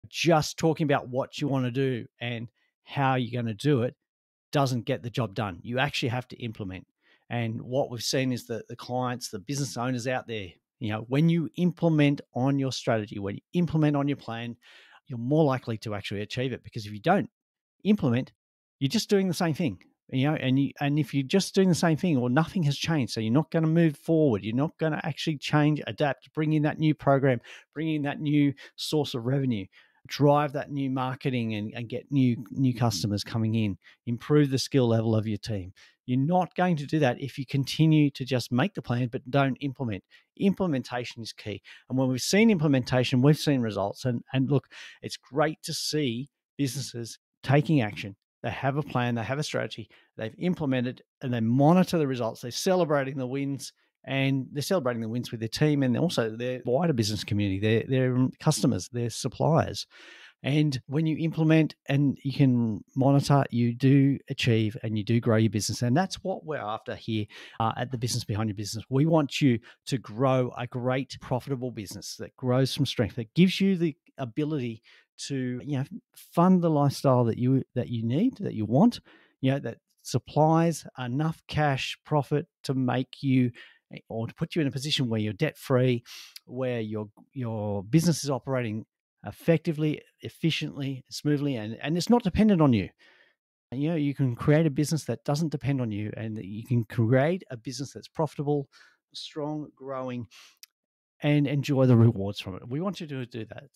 Bring Your Vision To Life: Master The Art Of Plan Implementation!
It's easy to make plans to improve your business - the hard part is actually doing the work to implement those plans. Many business owners struggle with this issue. They look at their business and see all of the things they want to change, then they come up with a great plan to improve their business - but that's as far as it ever goes. The plan stays a plan and the issues in the business continue to be a problem.

So make sure you take that key next step and implement your business plans.

Create manageable steps and do regular reviews to make sure you are getting the desired results.